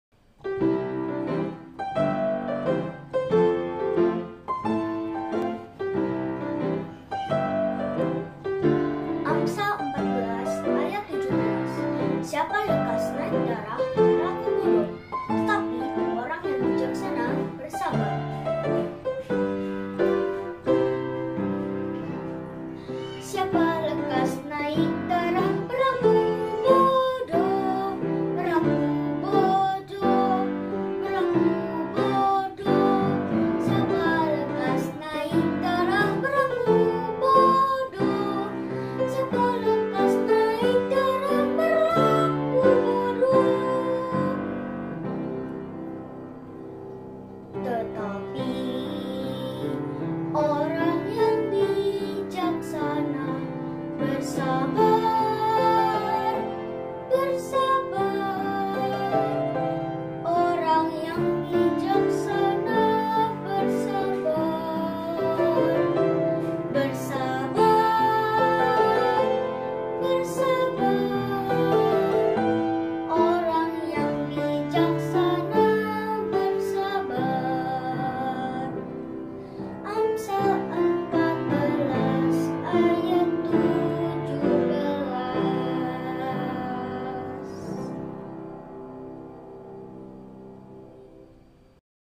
Amsal 14:17
Amsal 14:17 - "Siapa lekas naik darah berlaku bodoh, tetapi orang yang bijaksana bersabar."